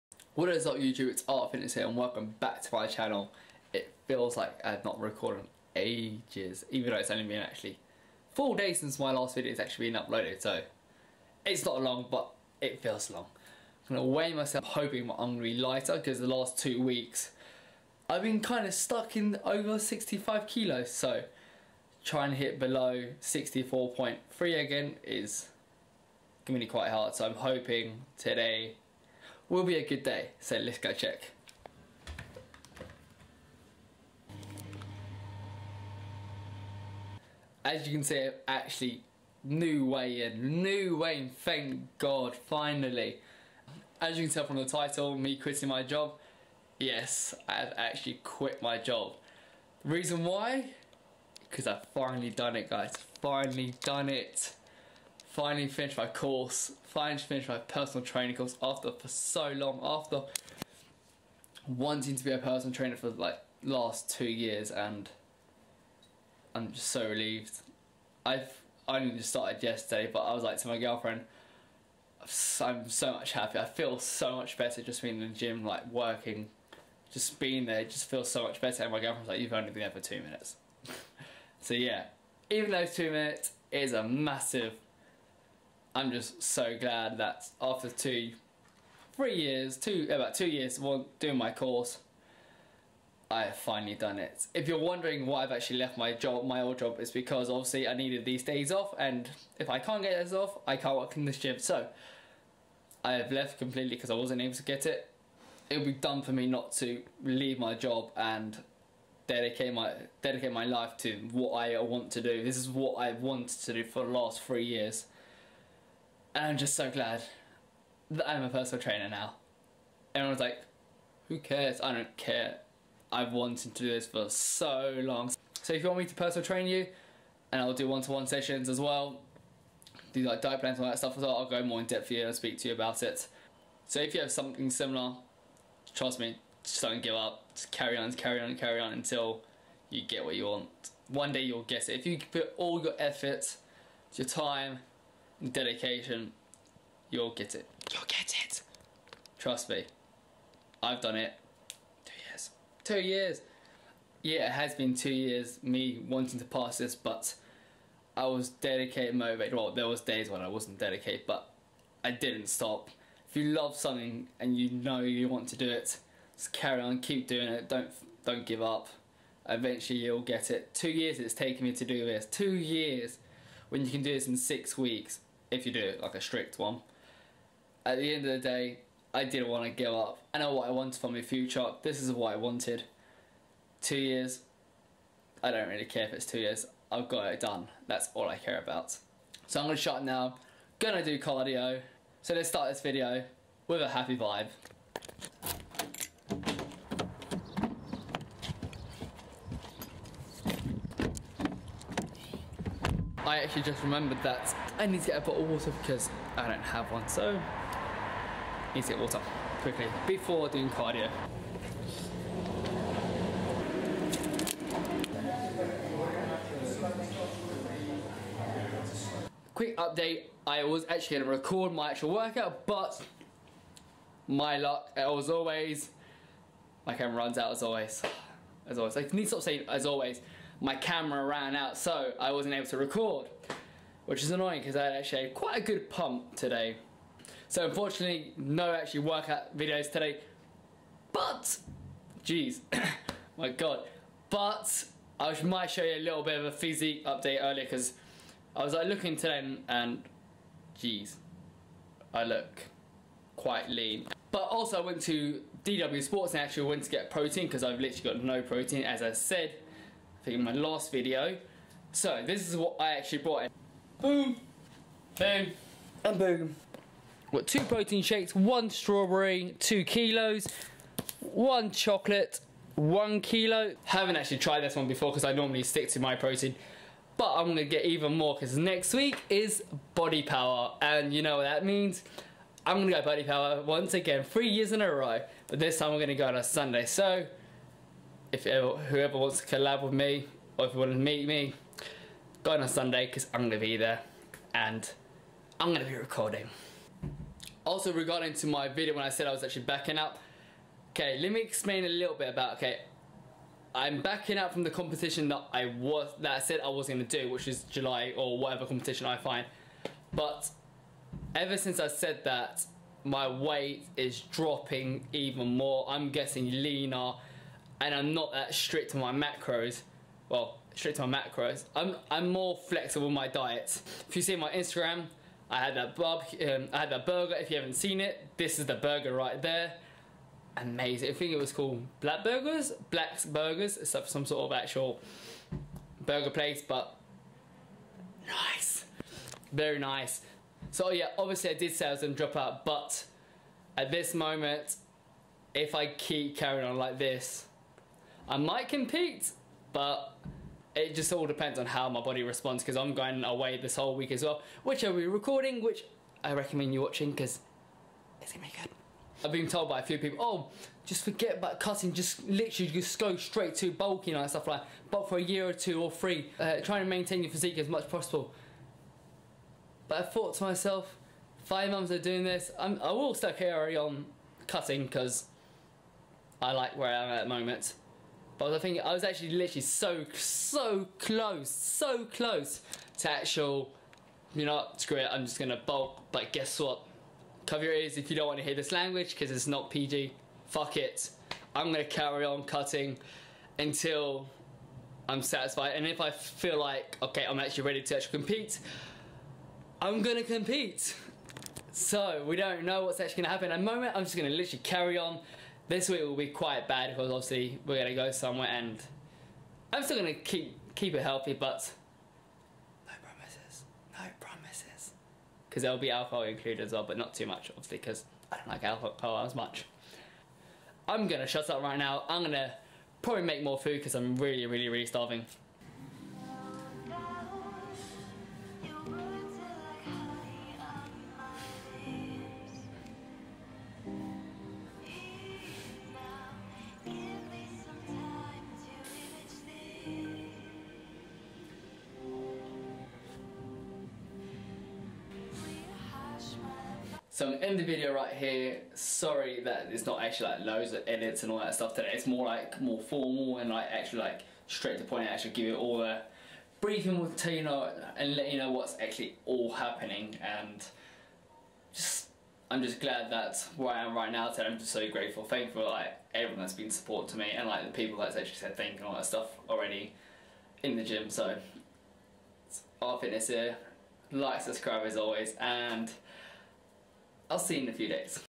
Quitting My Job.. | PHYSIQUE UPDATE | GETTING SHREDDED
A little physique update and speaking about my progress and my future goals.
This is one of my favourite video by far! im getting leaner by the day so will i actually compete at the end..